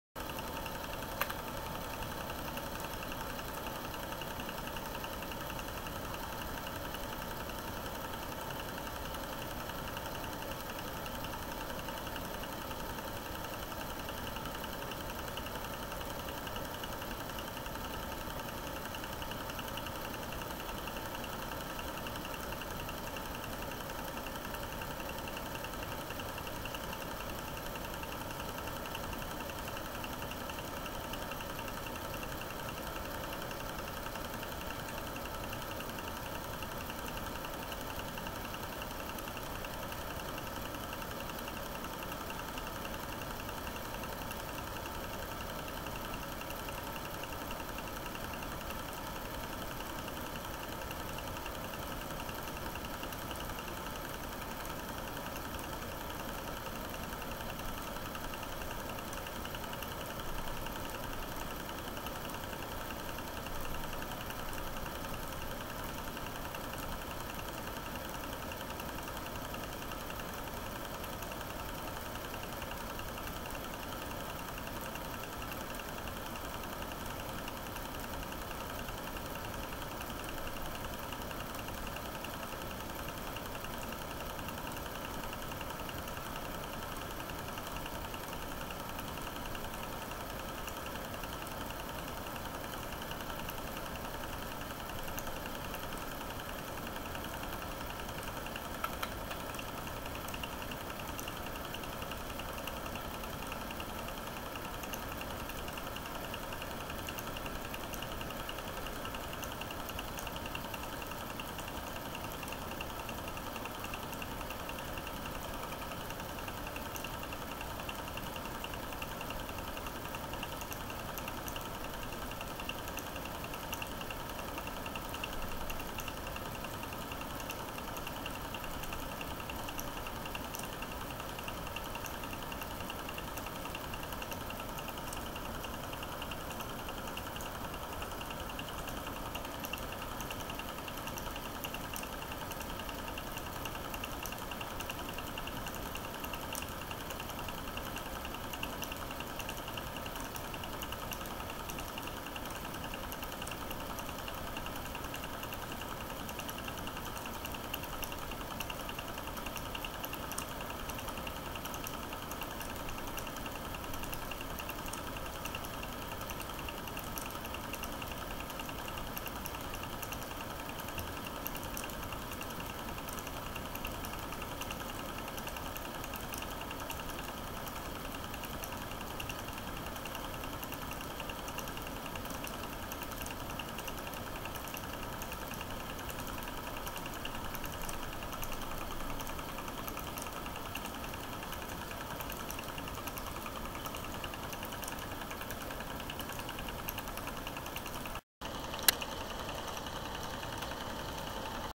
9.5mm Film, Scarborough,Oliver's Mount,Steam Train
9.5mm Film
Trafalgar Square, Scarborough Coastline/Harbour ??
York or Lincoln Cathedral ??
War Memorial, Oliver's Mount - Scarborough
Steam Train...